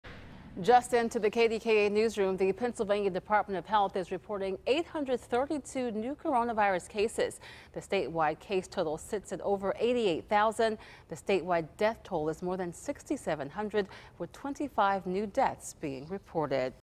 Coronavirus Cases Rise In Pennsylvania
KDKA breaks down the latest numbers from the state Department of Health.